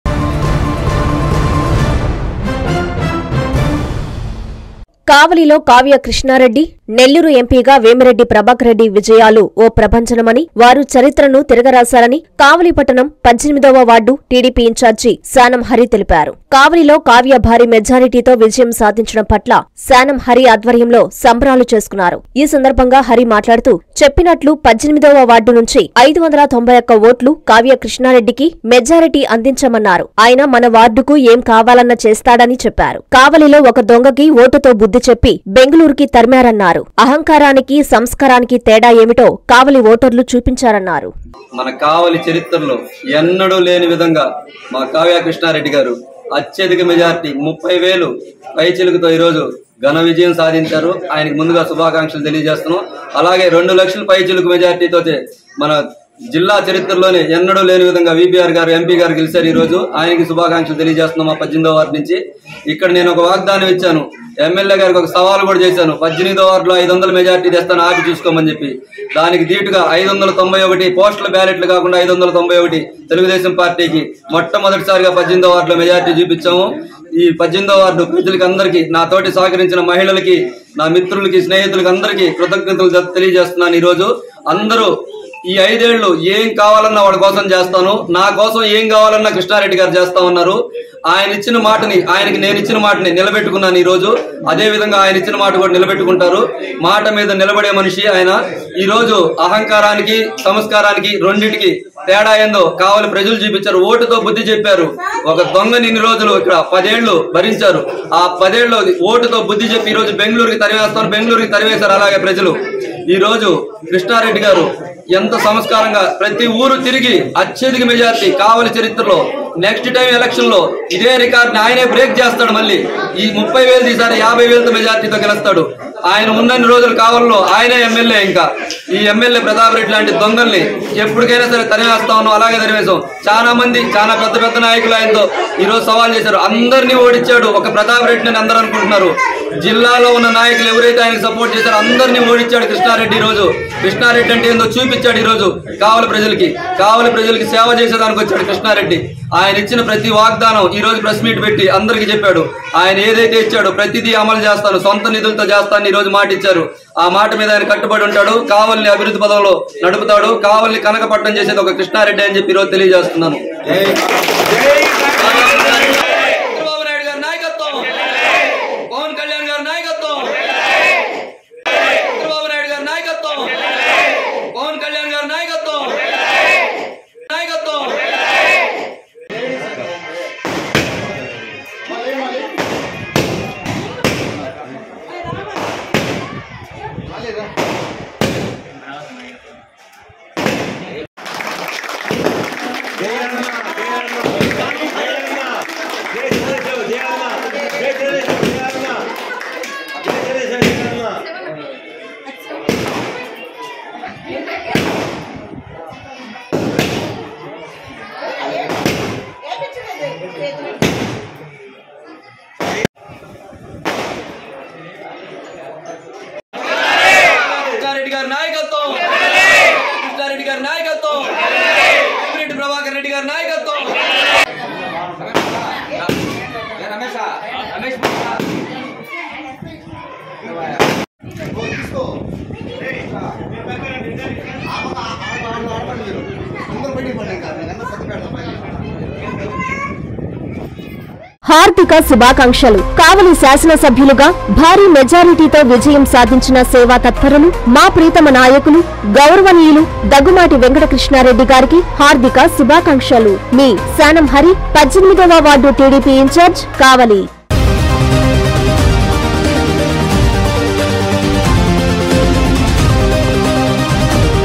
చరిత్రను తిరగరాశారు
- కావలిలో కావ్య కృష్ణారెడ్డి, నెల్లూరు ఎంపీగా వేమిరెడ్డి విజయం ఓ ప్రభంజనం
- చెప్పినట్లు 591 ఓట్లు కావ్యాకు మెజార్టీ అందించాం
- కావలి 18వ వార్డు టీడీపి ఇంచార్జీ శానం హరి ఆధ్వర్యంలో సంబరాలు